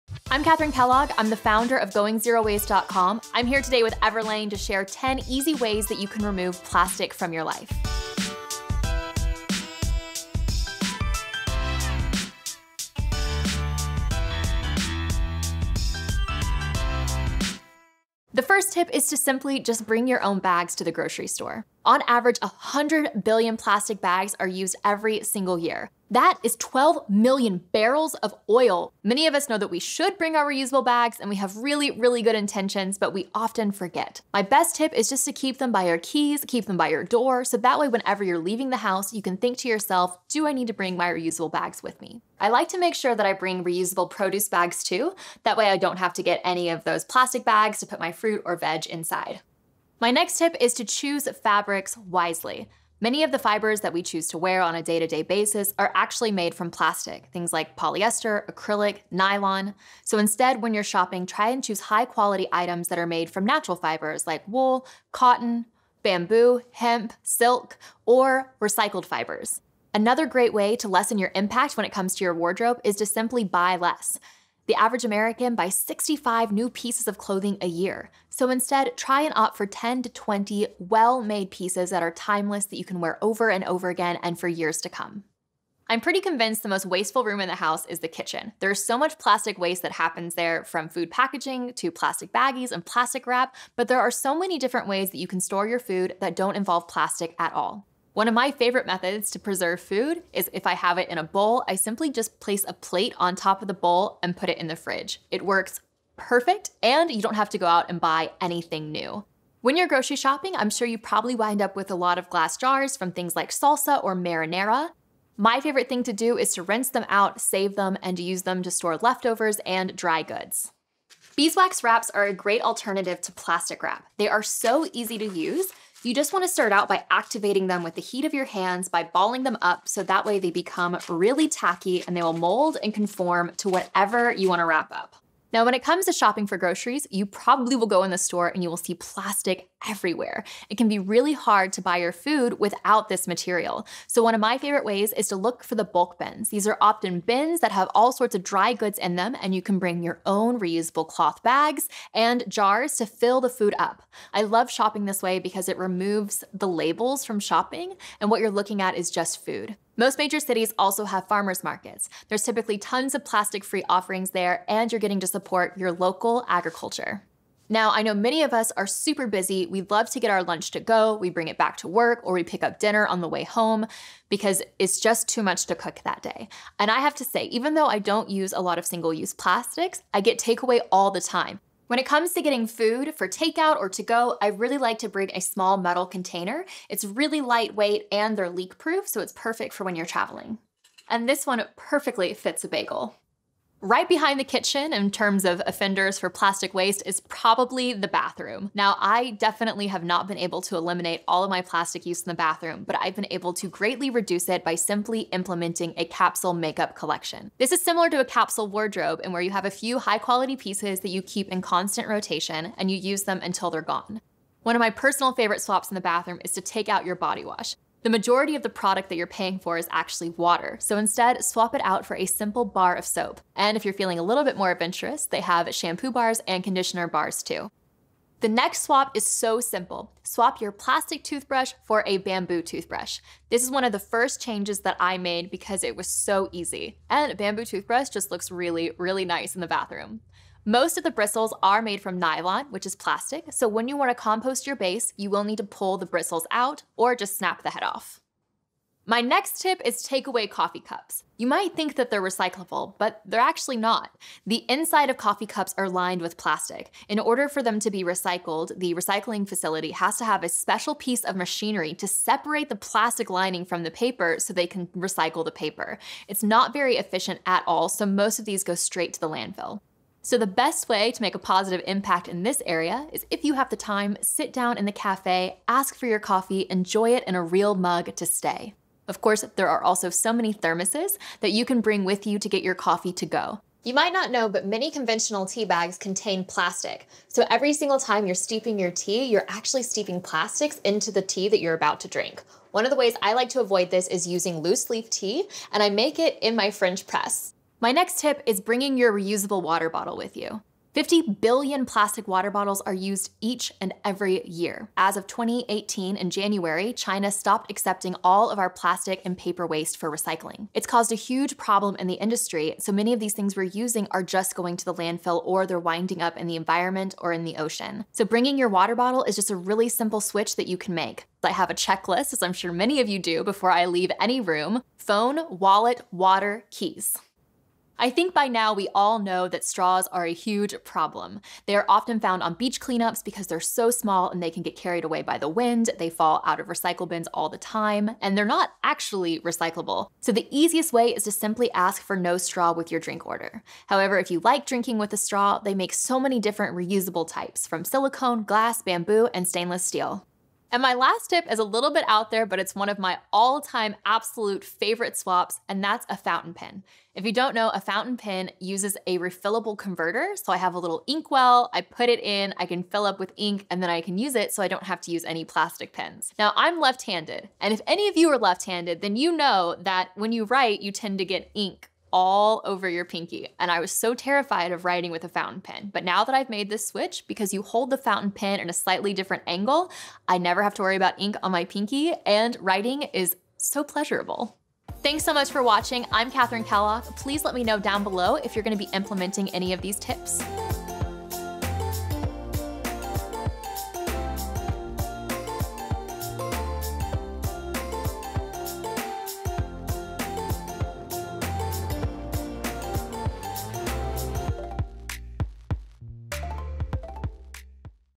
10 Easy Ways to Remove Plastic From Your Life | Everlane
Being eco-conscious never goes out of style. "Going Zero Waste" Founder Kathryn Kellogg shares some of her favorite ways to remove plastic from her life.

Learn more about "Going Zero Waste" and how you can do your part

Want to know more about these tips? Here are Kathryn's links and references:
Effects of Plastic Usage:

China Stops Accepting U.S. Recycling Waste